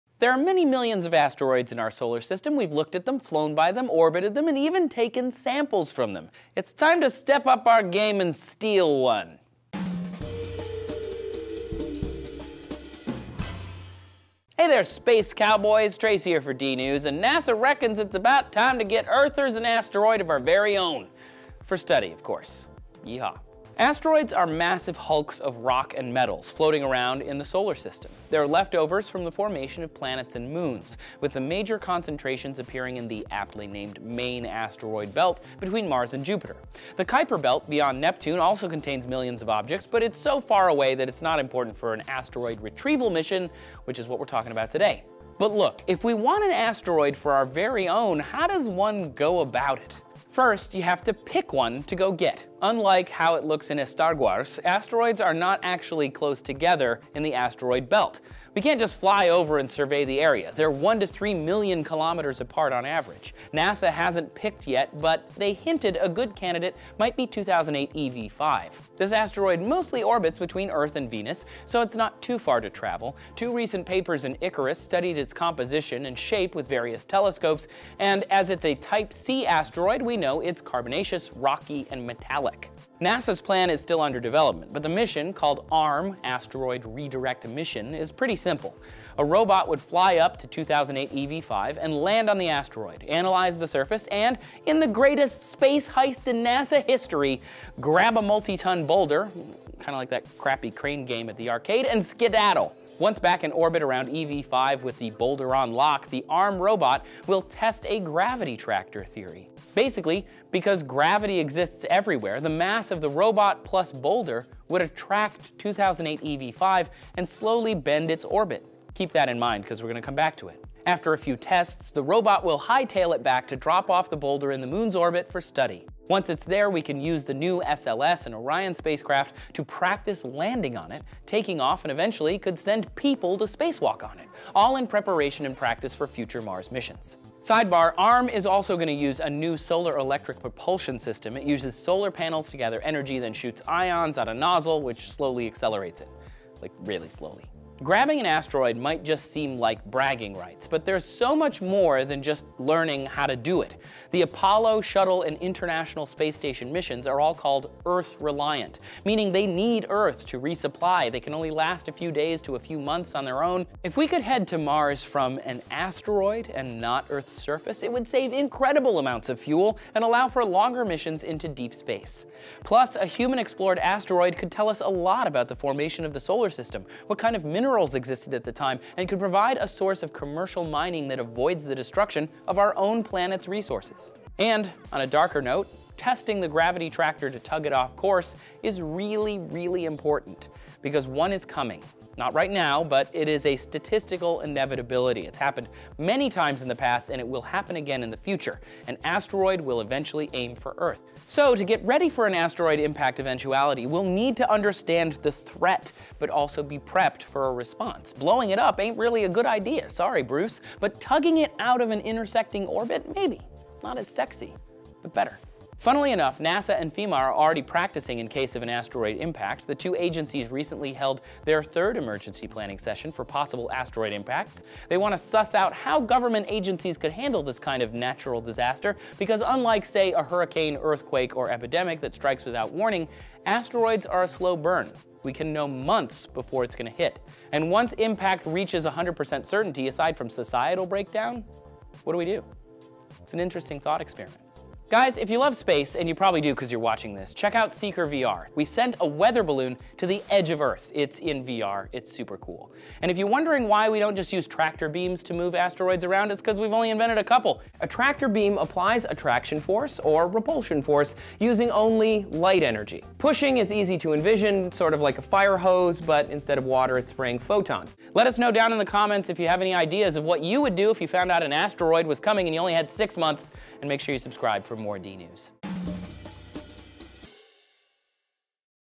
NASA's Plan To Give Our Moon Its Own Moon
NASA is planning on taking a piece of an asteroid and redirecting it into the moon's orbit, but how is that even possible?

What Is NASA's Asteroid Redirect Mission?
"NASA is developing a first-ever robotic mission to visit a large near-Earth asteroid, collect a multi-ton boulder from its surface, and redirect it into a stable orbit around the moon. Once it's there, astronauts will explore it and return with samples in the 2020s. This Asteroid Redirect Mission (ARM) is part of NASA's plan to advance the new technologies and spaceflight experience needed for a human mission to the Martian system in the 2030s."

How Will NASA's Asteroid Redirect Mission Help Humans Reach Mars?
"A human mission to and from the Mars system could last 500 days or longer, including six to nine months of transit each way. Missions to Mars will need to be "Earth Independent." To become Earth independent, NASA will develop and test through ARM a number of new technologies and capabilities that will directly enable future missions to Mars."

NASA's Asteroid Redirect Mission Emerges  From First Planning Stages
"NASA will not have to choose a target asteroid until 2020, but in the meantime the agency has focused on 2008 EV5 as a reference. This 1,300-foot-wide (400 meters) asteroid orbits the sun every 343 days. Observations with radio telescopes suggest that the asteroid has boulders of a variety of sizes that ARM could choose from."